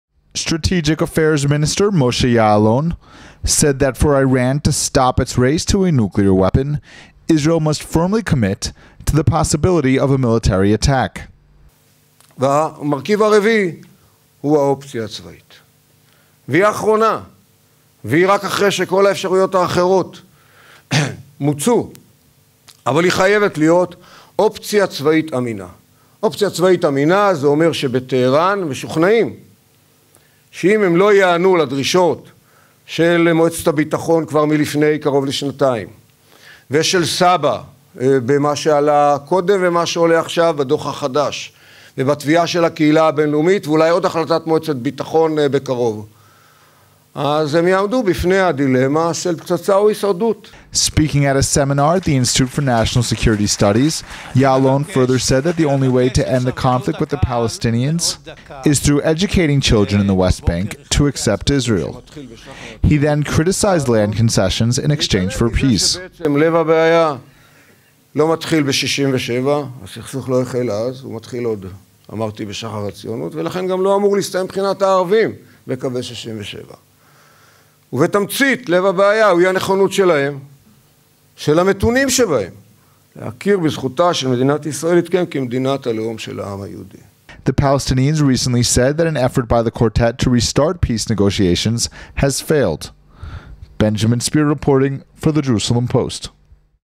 Ya'alon: Military option must be believable for Iran
Vice premier tells conference that an Israeli strike on Iran must be the last option, Palestinians must recognize Jewish state.